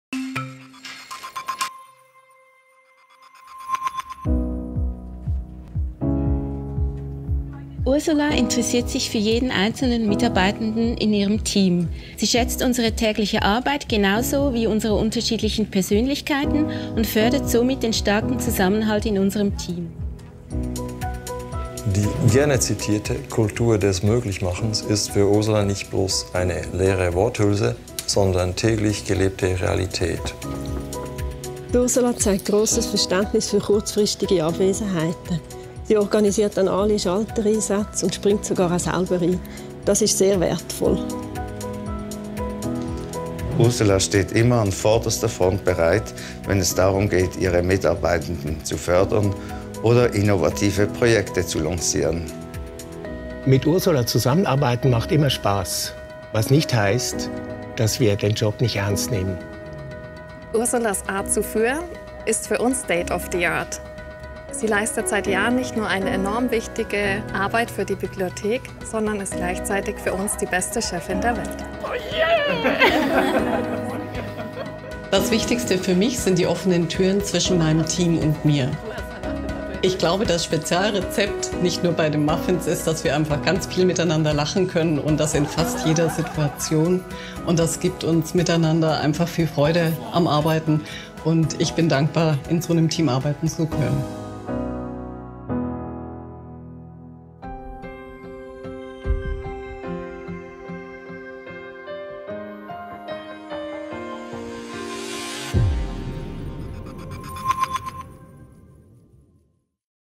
ALEA Award 2019 - Ursula Müller
Out of a total of 43 nominees for the first time three women were finalists for the ALEA Award in 2019:  Ursula Müller, head of the ETH library (ETH Zentrum), Consuelo de Moraes, Professor at the Department of Environmental Systems Science, and Gabi Hug, Professor at the Department of Information Technology and Electrical Engineering. Gabi Hug won the award for exemplary leadership.